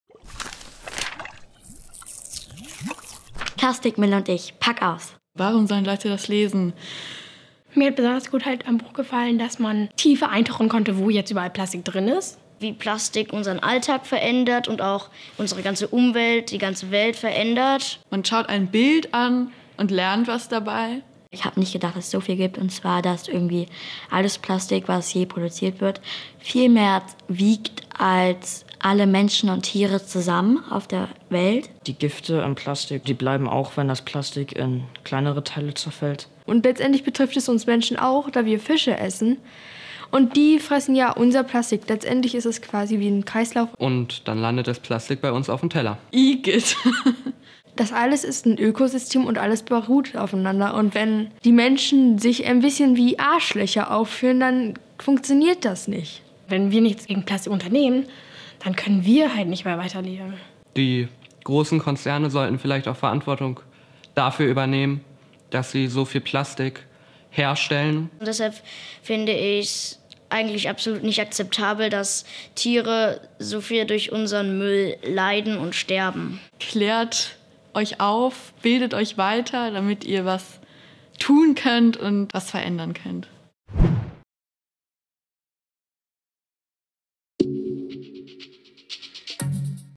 Pack aus! Plastik, Müll & ich
Mitglieder des Jugendbeirats, die am Buch mitgearbeitet haben, erklären, warum dieses neue Buch über Plastik so besonders ist, was das Problem mit Plastik ist und warum wir die Plastikflut dringend stoppen müssen.